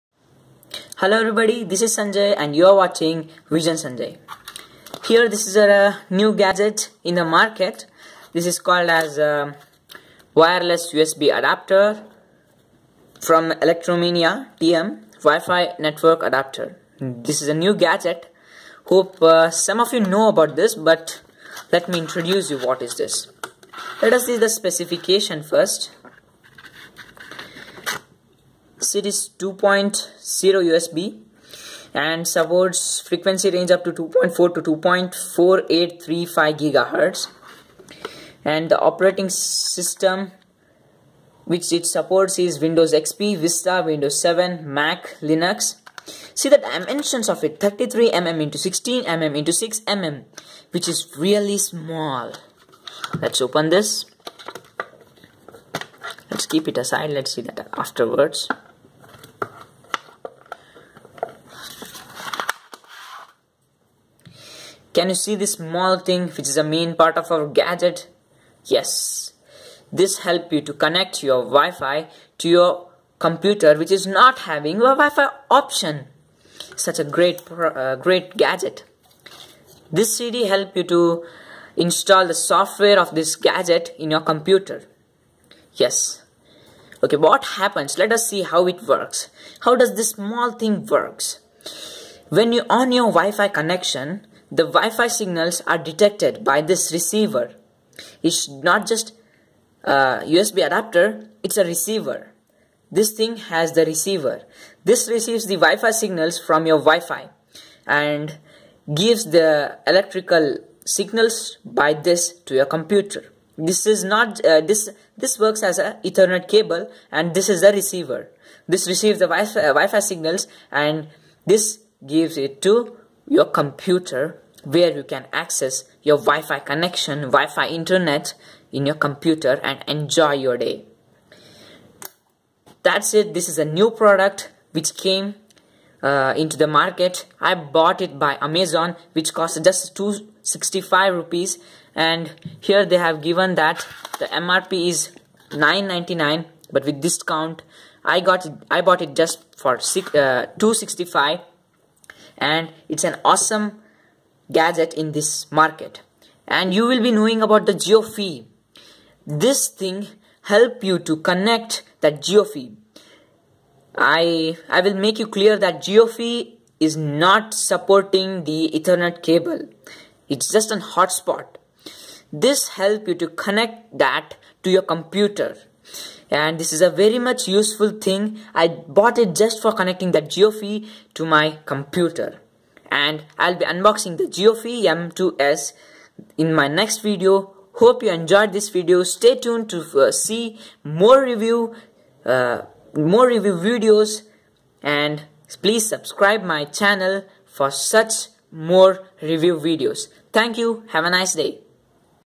Connect JioFi to ur non wifi PC | Best Wireless USB Adapter for PC | How to connect hotspot to PC
This is a new gadget.

Mini portable USB 2.0 design. This 300Mbps portable mini wireless USB adapter allows you to connect PC to a wireless network and access high-speed Internet connection. It's ideal for internet surfing and on-line gaming. Wireless N speed up to 300Mbps, ideal for internet surfing and on-line gaming. Complies with IEEE 802.11n (Draft 2.0), IEEE 802.11g, IEEE 802.11b standards. Provides two work modes: Infrastructure and Ad-Hoc. MIMO technology makes wireless transmission rates up to 300Mbps. CCA technology enhances your signal stablility by automatically avoiding channel conflicts. Quick secure setup, complies with WPS for worry free wireless security. Wireless security encryption easily at a push Of QSS button. Supports 64/128-bit WEP, as well as WPA/WPA2 and WPA-PSK/WPA2-PSK encryptions. Supports WIN2K, XP, VISTA, WIN7,LINUX operating system. Supports wireless roaming technology for high-efficient wireless connections. Seamlessly compatible with 802.11b/g/n devices.